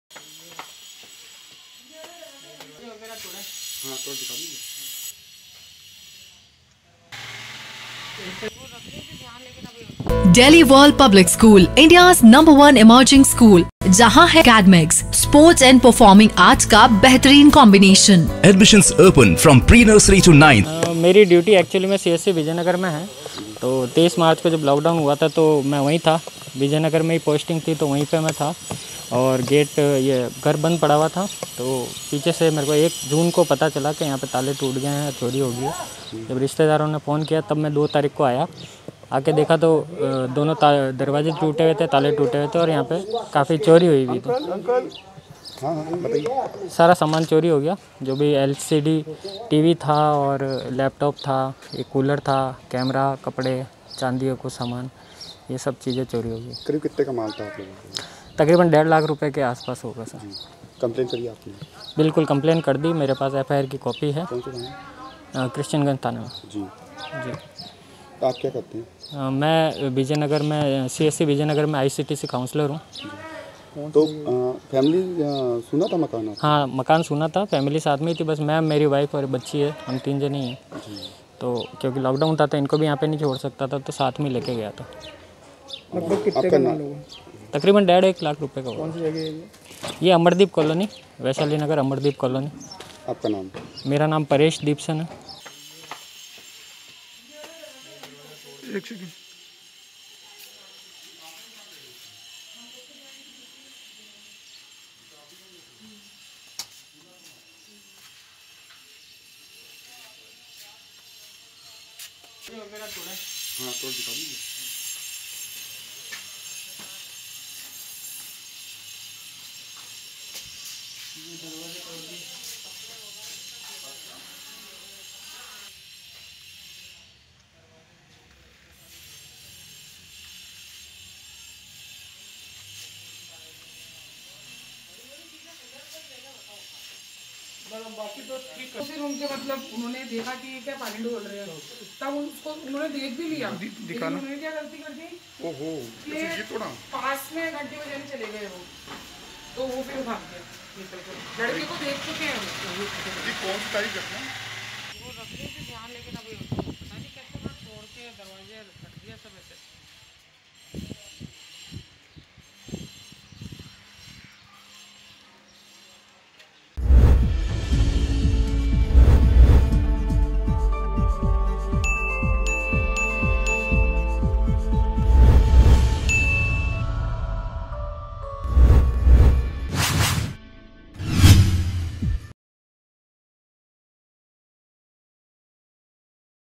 Ajmer | क्रिश्यनगंज थाना इलाके के अमरदीप कॉलोनी वैशाली नगर में लाखों की चोरी | MTTV INDIA
अजमेर क्रिश्यनगंज थाना इलाके के अमरदीप कॉलोनी वैशाली नगर में लाखों की चोरी
घर का ताला तोड़कर नगदी व सामान लेकर चोर फरार
करीब एक लाख रुपए का माल लेकर चोर हुआ फरार
पीड़ित ने क्रिश्यनगंज थाने में मुकदमा दर्ज करवाया